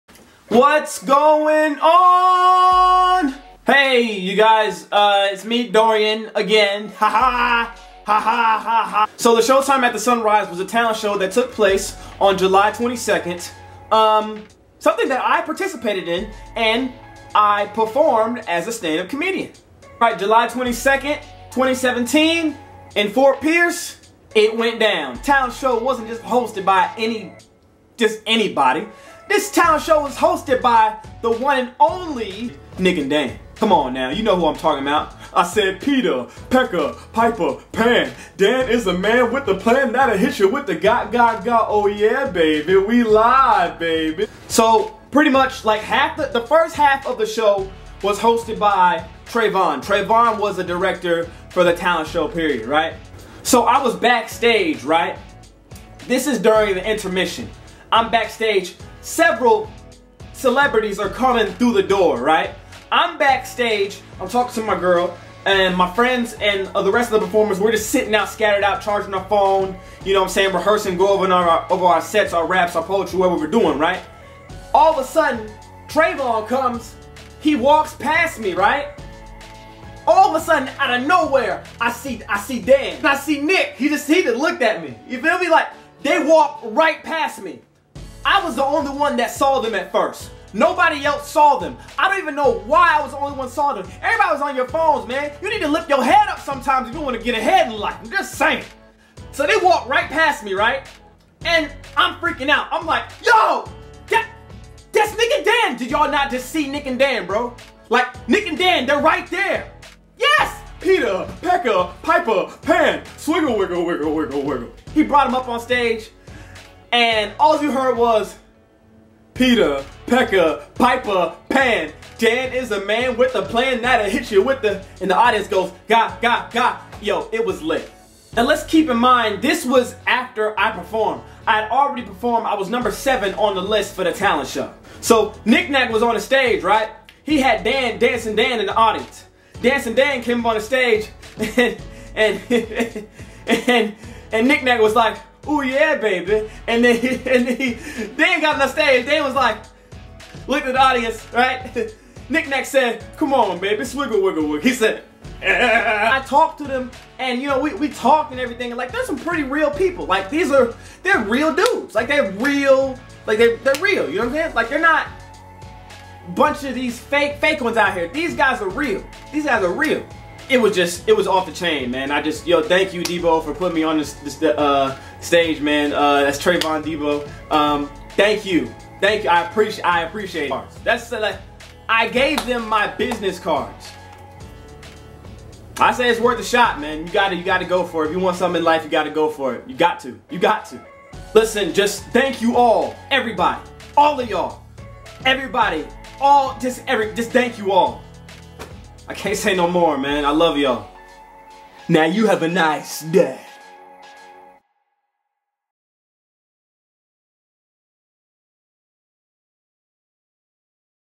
HOW I MET NICK AND DANRUE!!!
Showtime at The Sunrise Recap
This video is all about how I met the famous Nick and Danrue
It was amazing time so listen to this short story on how I met these two entertainers.